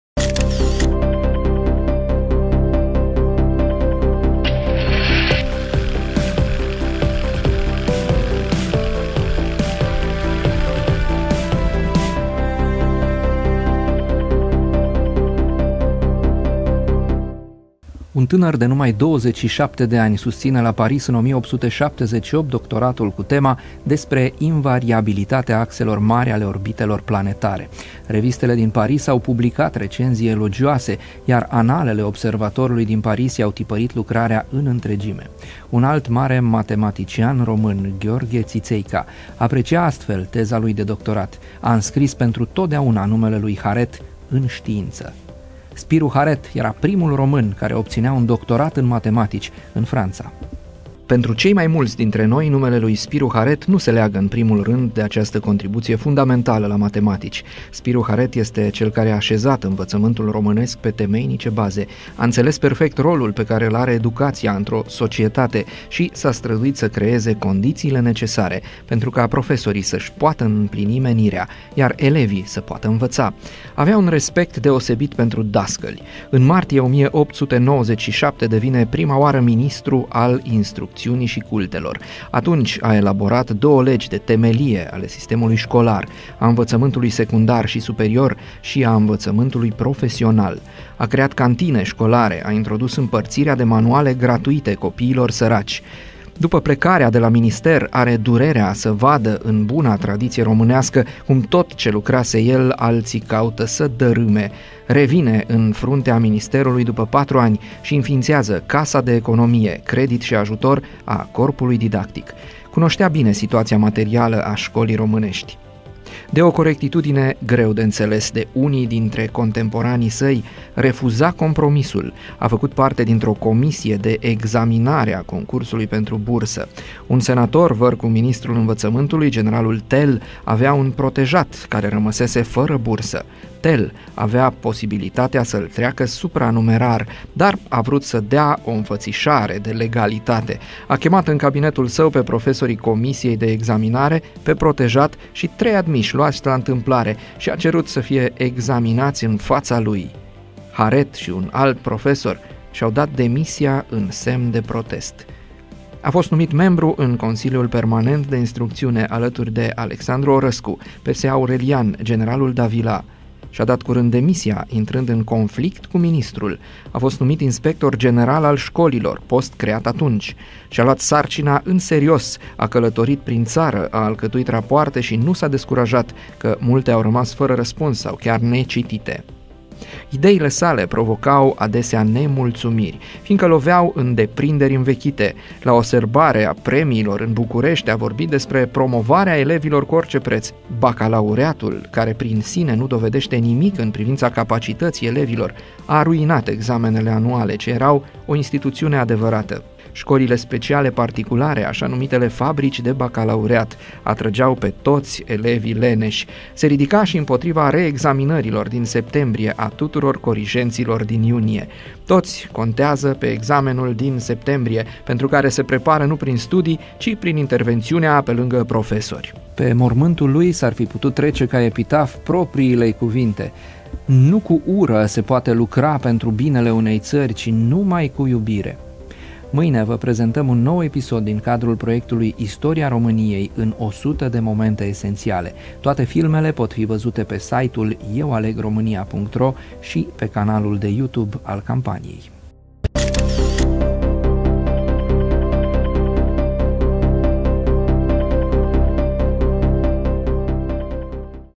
Spiru Haret - cel care a aşezat învăţământul românesc pe temeinice baze
La 30 ianuarie 1878, un tânăr de numai 27 ani, susţine la Paris doctoratul cu tema Despre invariabilitatea axelor mari ale orbitelor planetare. Revistele din Paris au publicat recenzii elogioase, iar Analele Observatorului din Paris i-au tipărit lucrarea în întregime, în 1885. Un alt mare matematician român, Gheorghe Ţiţeica, aprecia astfel teza lui de doctorat: „A înscris pentru totdeauna numele lui Haret în ştiinţă“. Spiru Haret era primul român care obţinea un doctorat în matematici în Franţa. Era cât pe-aci să nu mai ajungă să îl susţină. În aprilie 1877 intrasem în război alături de Rusia contra Imperiului Otoman. Bursierii români din Paris au socotit de datoria lor să vină în ţară, să se înroleze, dar ministrul Învăţământului, G. Chiţu, le-a telegrafiat să rămână şi să-şi continue studiile.

Consultant științific – Dorin Matei, redactor-șef Magazin istoric

Radio România Timișoara
Prezentare, voice over – Sergiu Galiș
Operator video/editor video – Ovidiu Novac
Coordonator proiect – Aurelian Teculescu